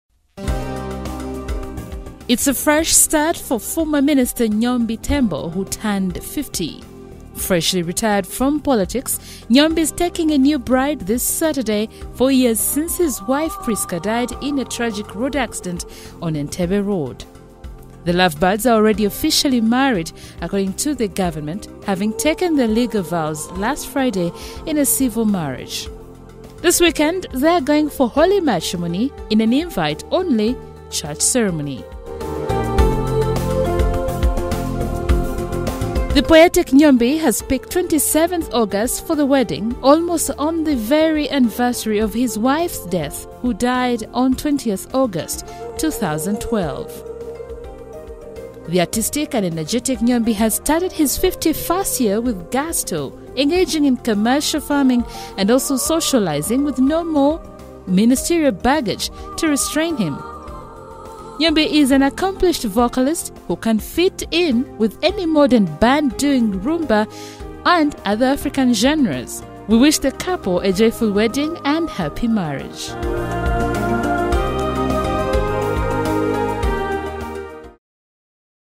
NYOMBI LOVES AGAIN
A flowery affair, an aura of taste and class as George William Nyombi  Thembo, former State Minister for ICT wedded his longtime companion, Angella Naggayi the mother of his two children in a civil wedding at colourful ceremony